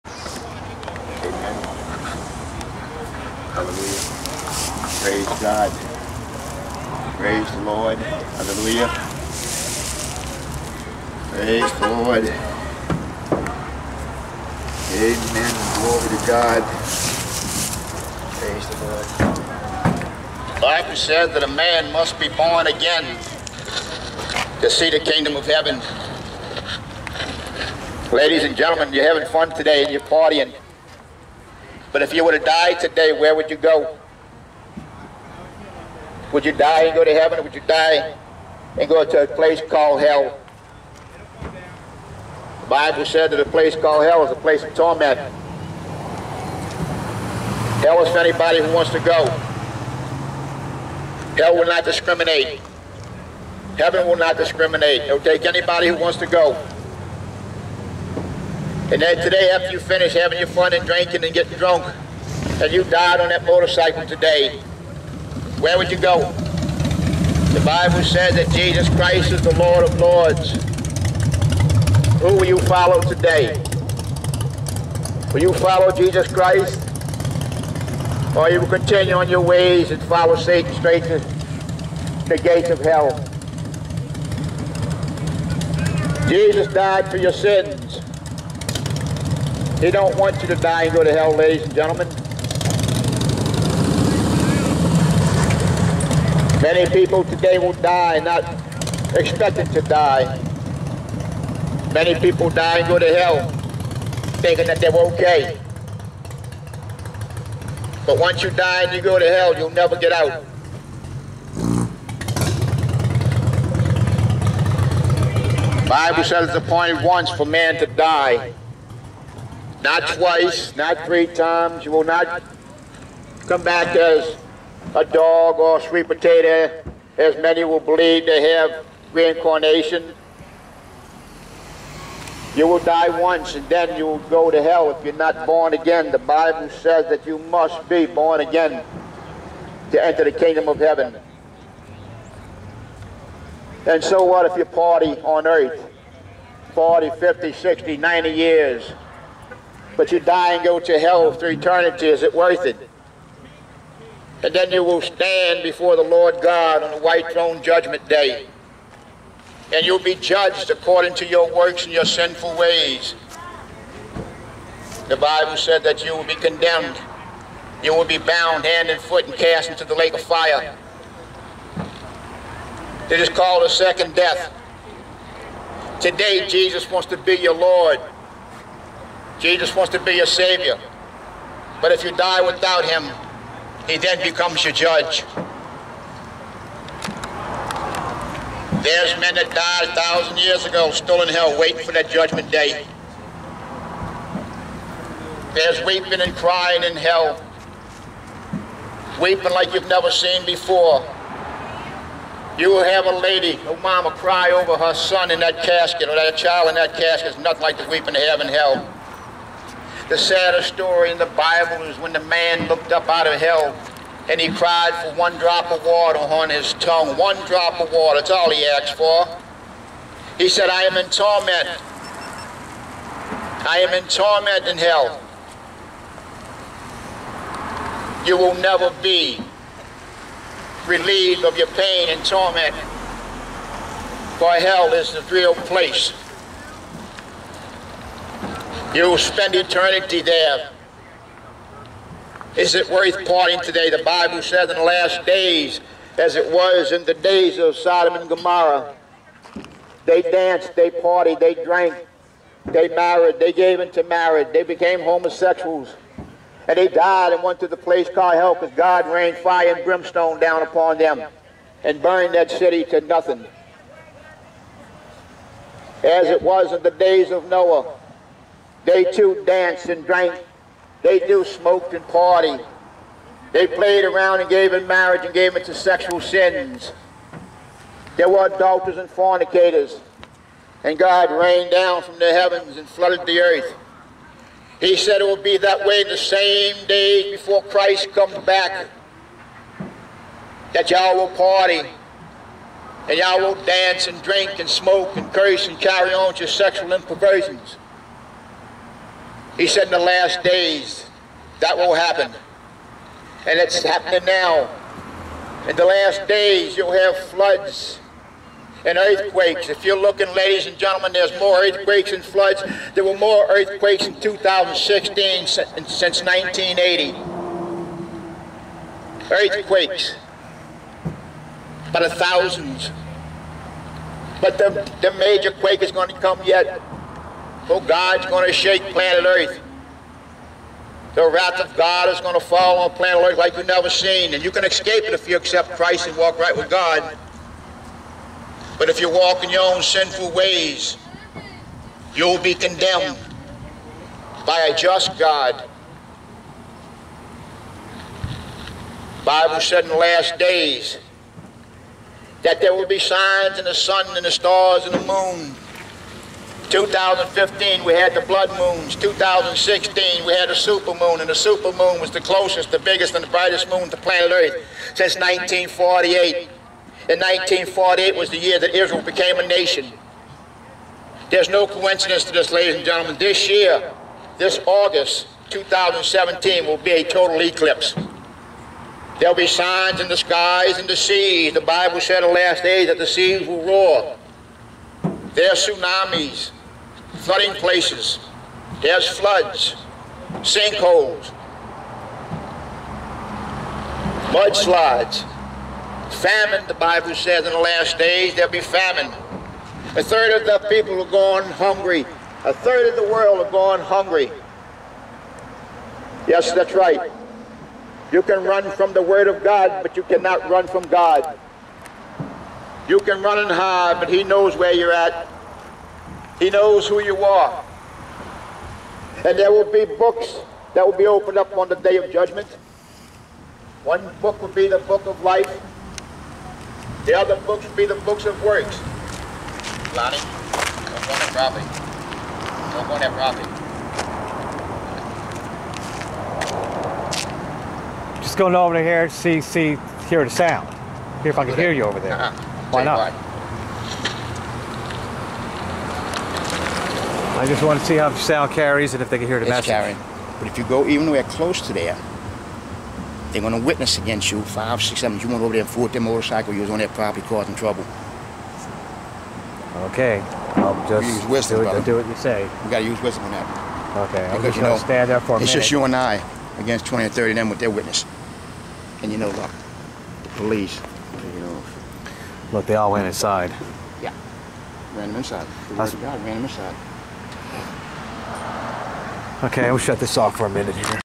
Hell Fire Street Preacher vs. Louisiana Biker Bar
Evangelist jimmy Miller preaching the Gospel of Jesus Christ at a biker bar. Lonnie Matherne is filming.